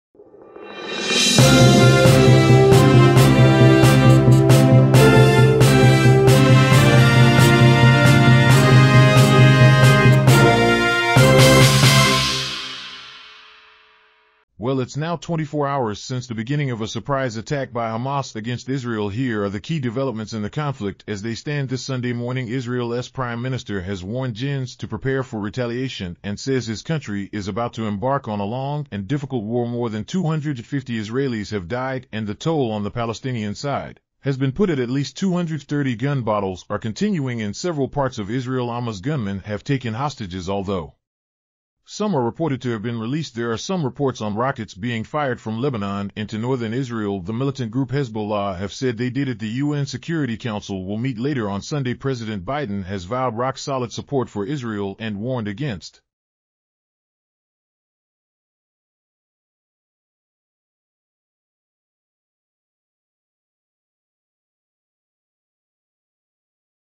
Hamas-Israel Conflict: 24 Hours of Escalation, International Response, Tensions Mount" BBC Ai News
This news update delves into the rapidly evolving conflict between Hamas and Israel, which has unfolded over the past 24 hours. Israel's Prime Minister has issued a stern warning of retaliation and the prospect of a protracted and challenging war. Casualties have risen, with over 250 Israelis and at least 230 Palestinians reported dead. Gun battles persist across multiple Israeli regions, with reports of hostages taken and even rocket fire from Lebanon attributed to Hezbollah. The global community's response is critical, with President Biden pledging solid support for Israel while cautioning against opportunism. The UN Security Council will convene an emergency meeting to address this deepening crisis. The situation in Gaza remains tense, with renewed airstrikes adding to the suffering. International cooperation, involving the United States and regional actors, is underway to mitigate the violence and restore stability.